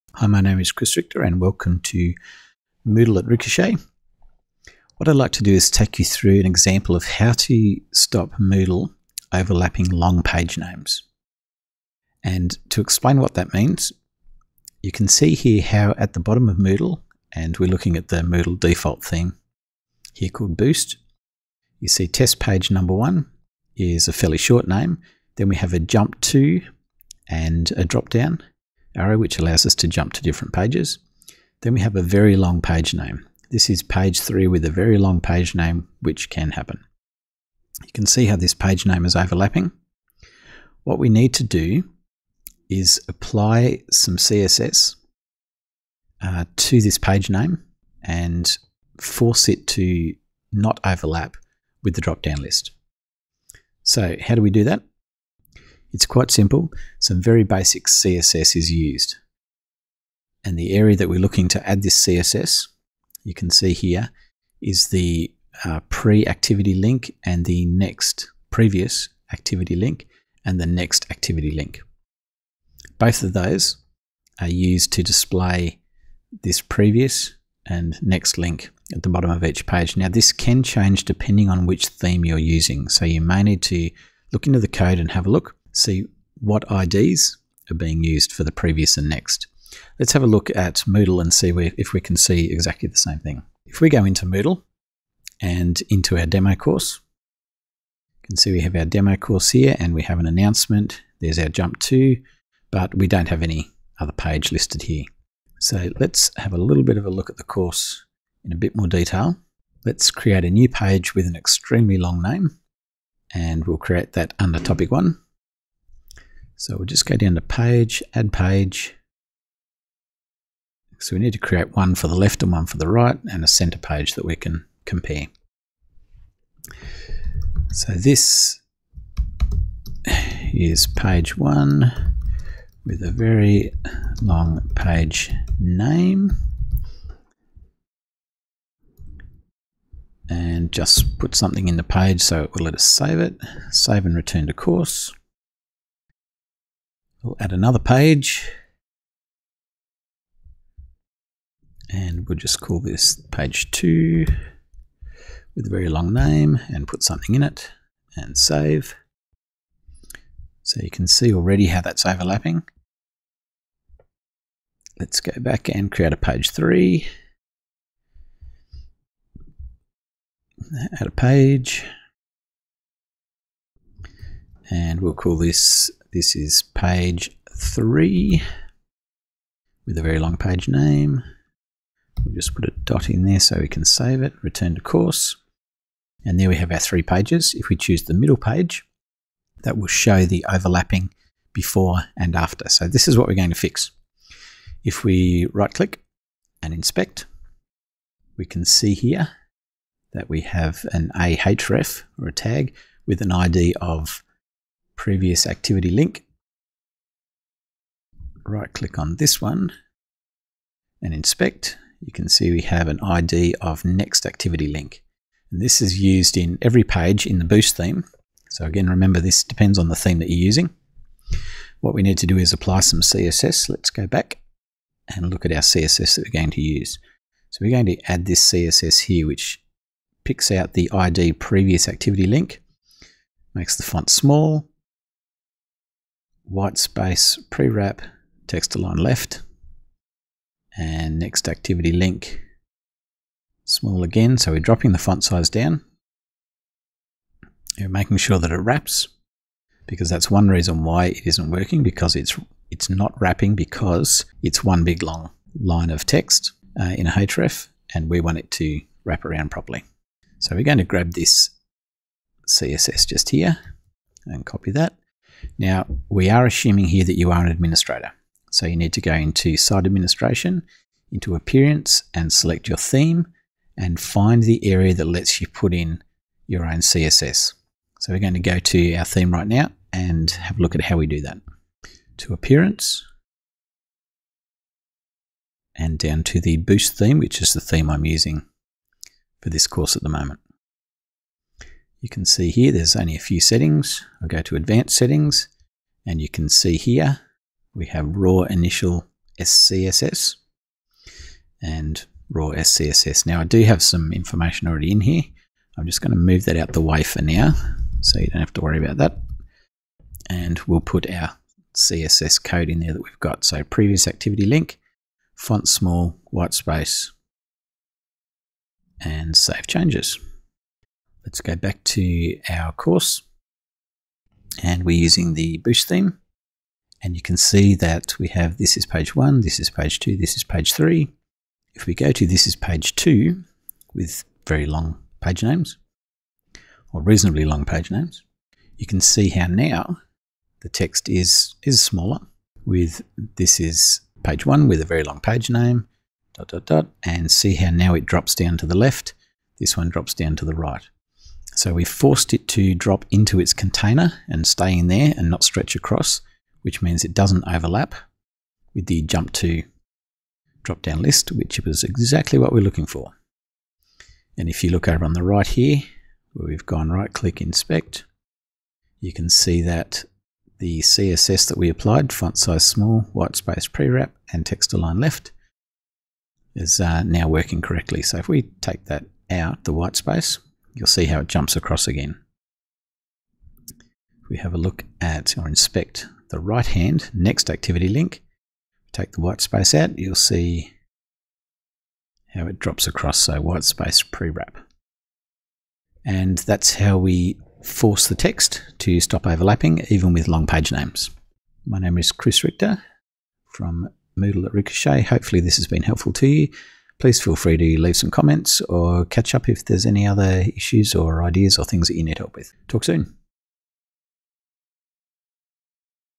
How to stop MOODLE NAVIGATION OVERLAPPING long page names - Ricoshae
How to stop Moodle navigation overlapping long page names - Tutorial